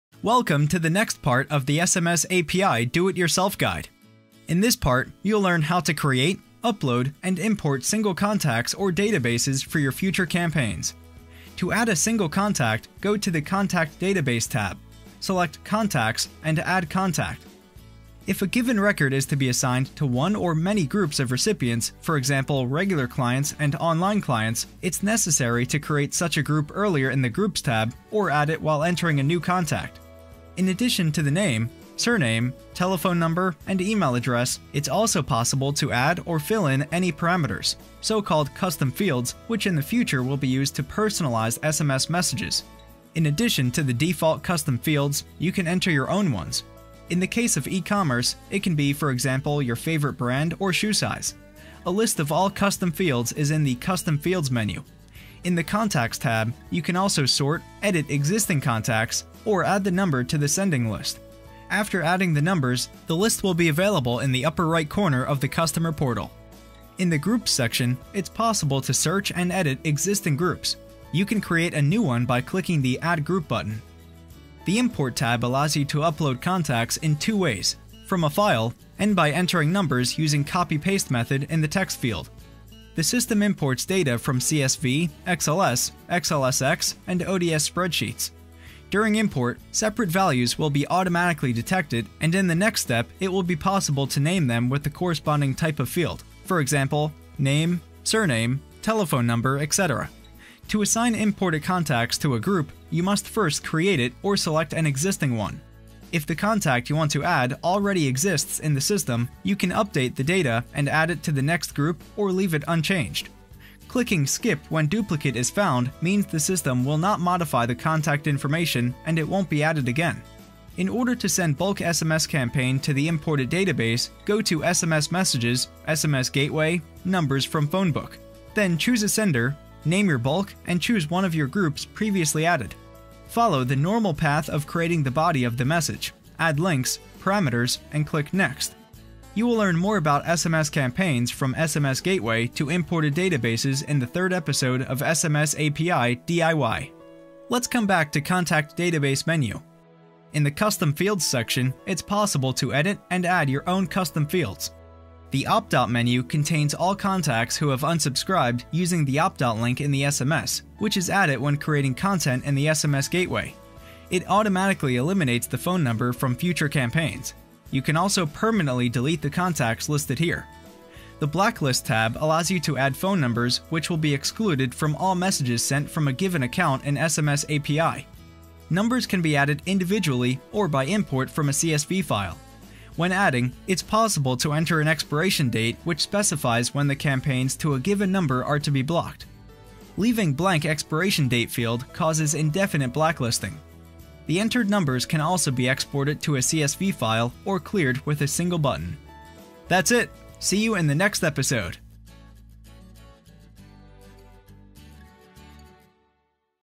Contact Database | SMSAPI. Do It Yourself!
Welcome to the series of video guides "SMSAPI. Do it yourself"!
In this part you will learn how to create, upload and import single contacts or databases for your future campaigns.